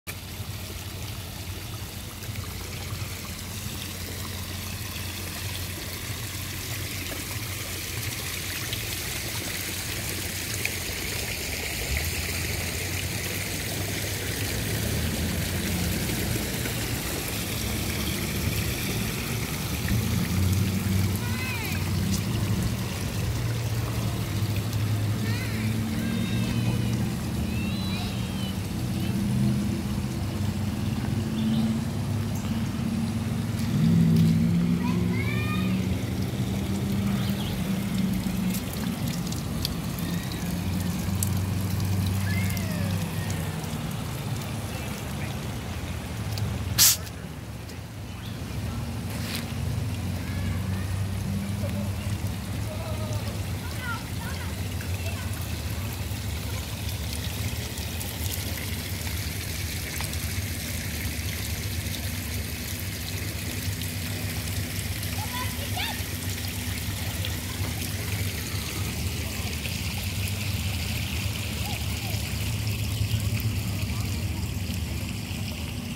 Overflowing
The hot water display in Arlington Park was overflowing